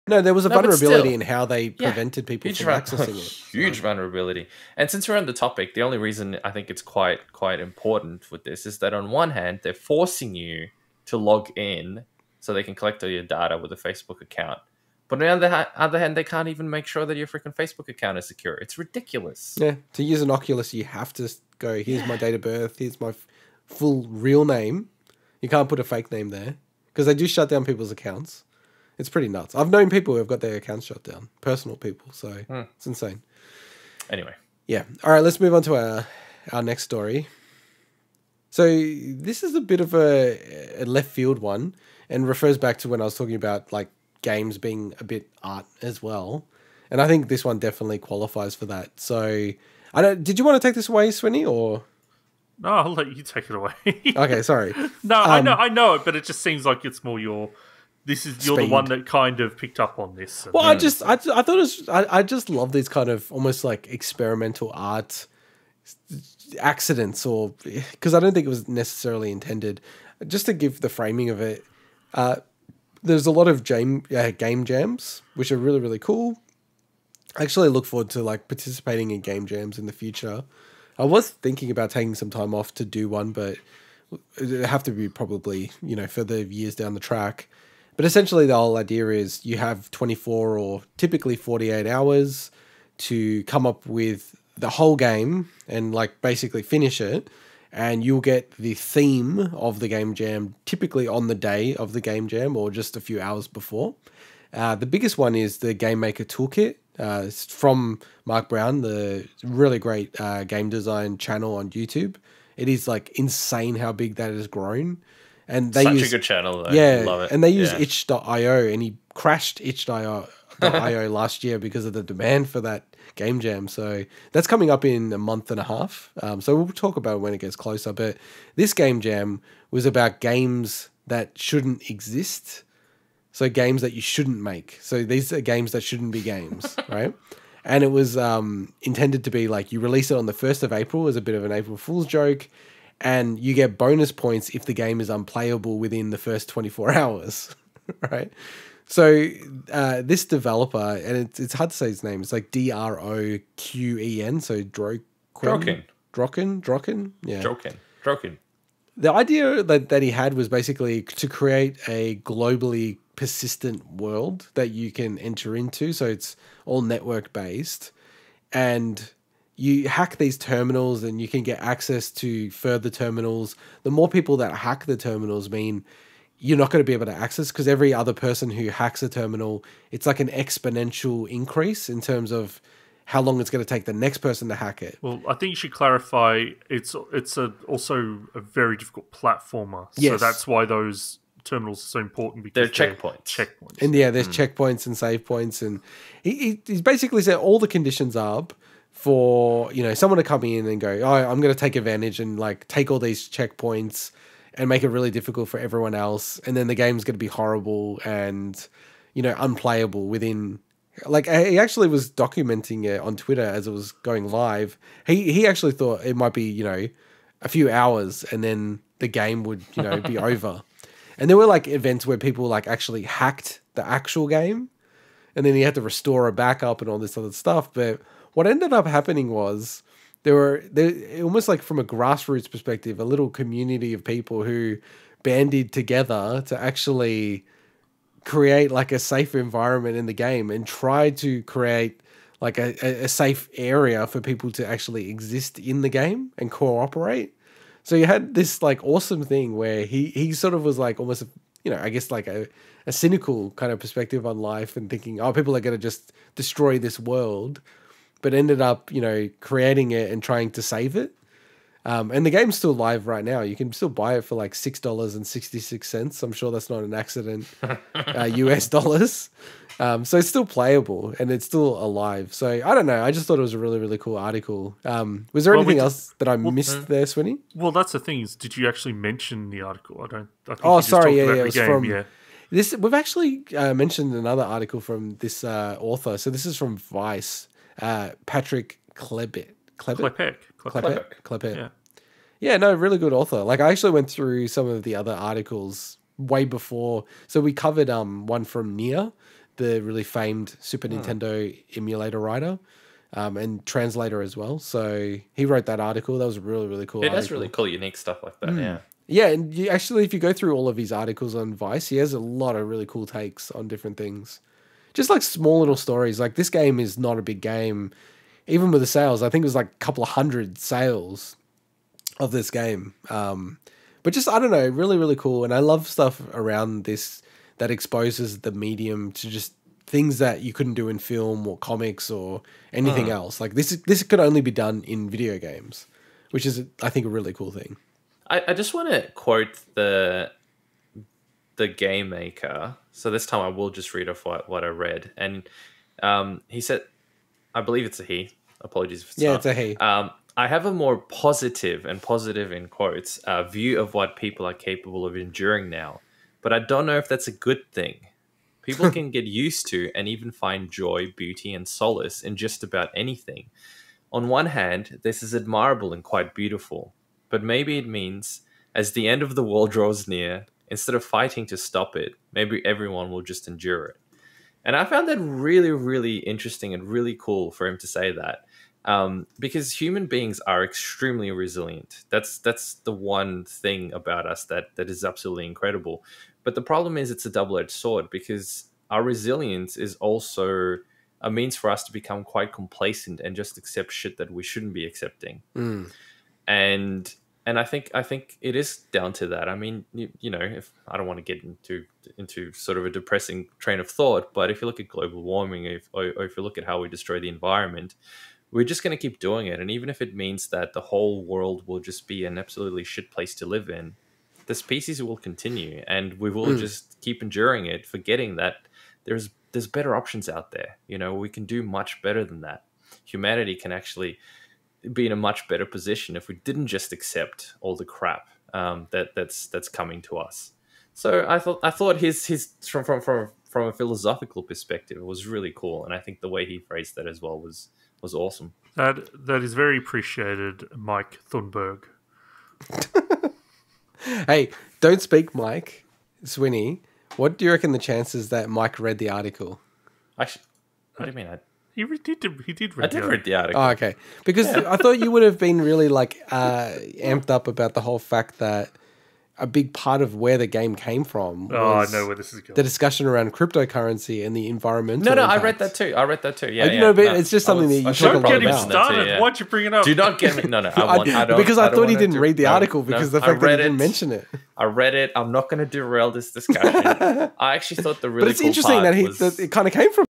Cruel World - How Players Are Trying To Save An Unplayable Game
FULL VIDEO PODCAST ▼

DESCRIPTION ▼
VICE's Patrick Klepek has published a really cool look at the indie game Cruel World - and how players have subverted the expectations of its creator.

Developed by Droqen, Cruel World is a 'masocore platformer' with a persistent game world, where players can take selfish actions to make the game miserable to play for everybody else. Throughout the levels are terminals that act as save points. However, these reset if you leave the game. Players can 'hack' these terminals to allow for future immediate access. But in doing so, it makes the hacking process more difficult for every other player after that (upwards of 15 minutes). After releasing on 1 Apr 2021, the expectation was that the game would essentially be unplayable after the first 24 hours, and Droqen would remove the game from sale. The community has defied expectations, with players taking philanthropic approaches to playing the game, roleplaying, working together, modding, etc. The experience has left Droqen with a more positive view of what players are willing to endure.